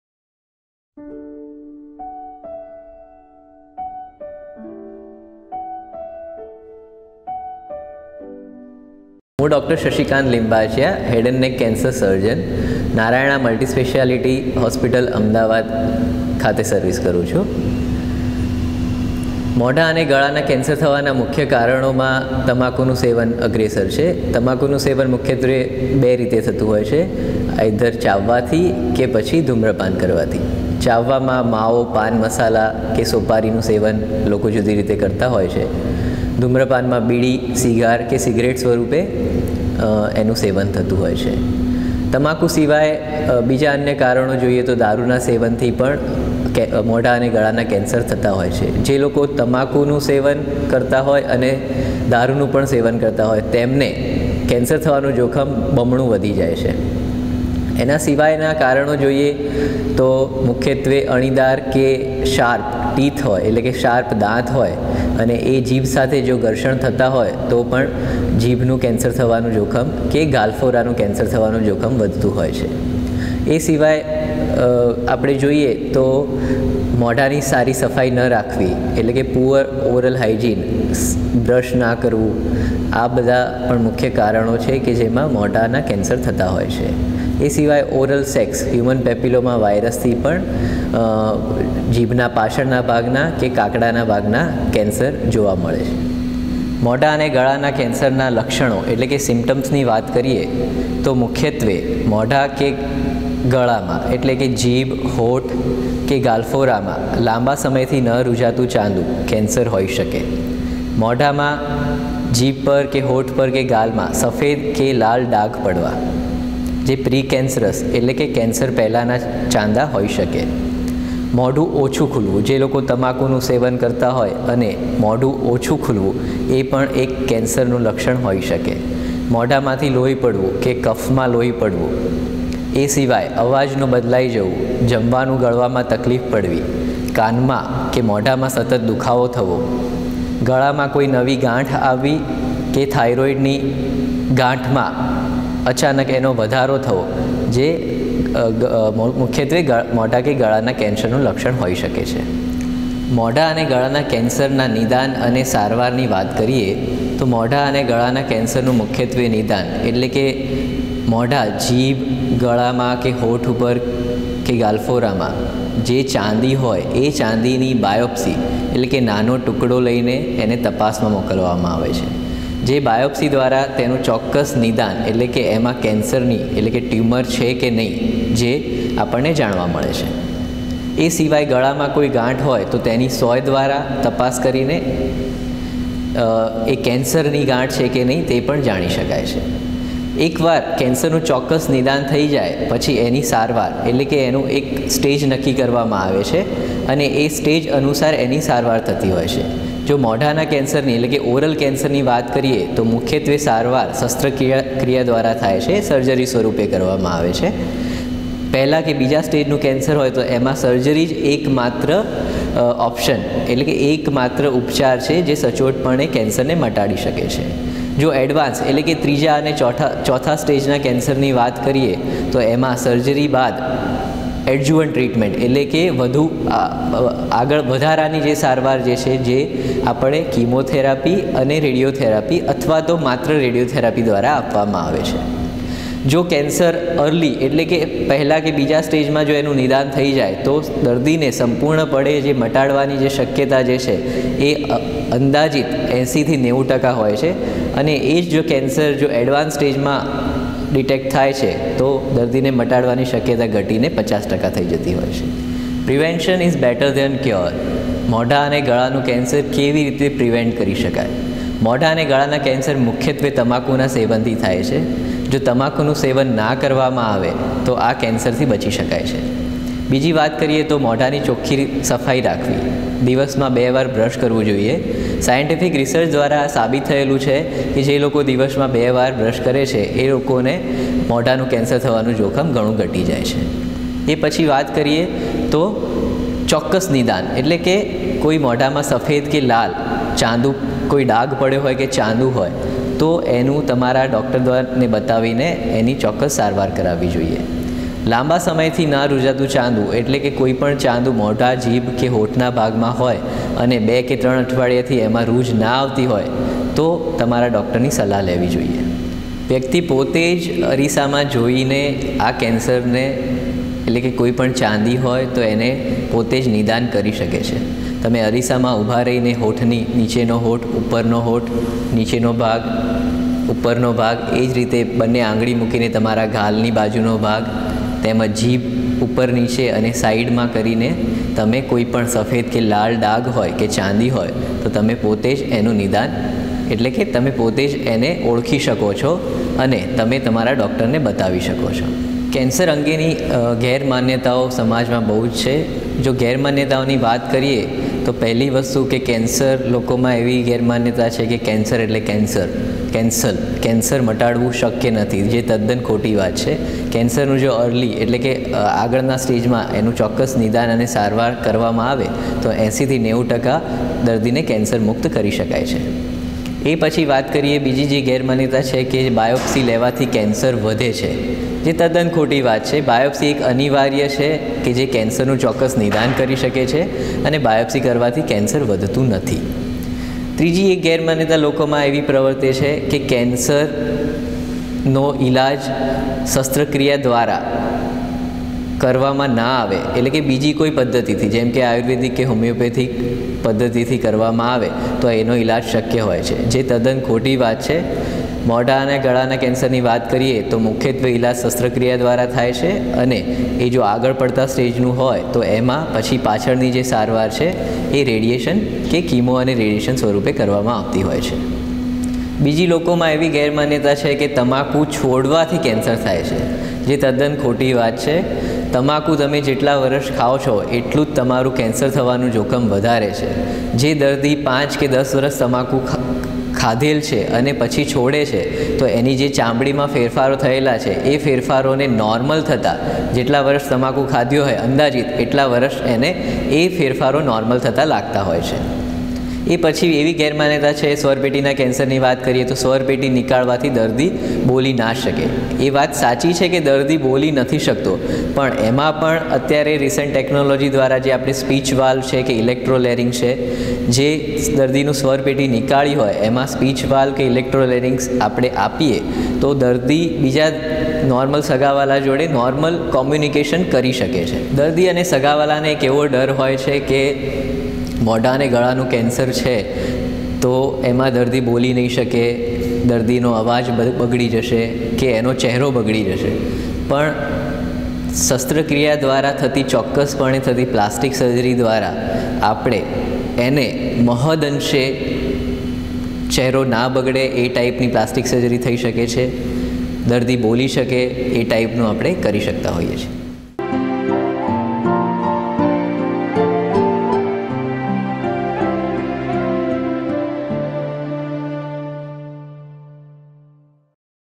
Head & Neck Cancer: Causes, Symptoms, Treatment & Prevention Explained
Head and Neck Cancer is a serious condition often caused by tobacco consumption, smoking, alcohol use, and other environmental factors. The early symptoms can include changes in voice, difficulty swallowing, or persistent sore throat. With early detection, treatment options can significantly improve the chances of recovery. Simple precautions and lifestyle changes can also help in preventing this cancer.

In this video, Dr. Shashikant Limbachiya, Head & Neck Cancer Surgeon at Narayana Multispeciality Hospital, Ahmedabad, discusses the common causes, symptoms, and treatment options for Head, Neck, Throat, and Mouth cancers. He also emphasizes the importance of early detection and how prevention strategies can lower the risk of these cancers.

If you suspect any symptoms or have concerns about Head and Neck Cancer, this video provides essential information and expert advice. Don't hesitate to talk to your doctor if you notice any unusual changes.

For more information about Head and Neck Cancer treatment and services, visit Narayana Health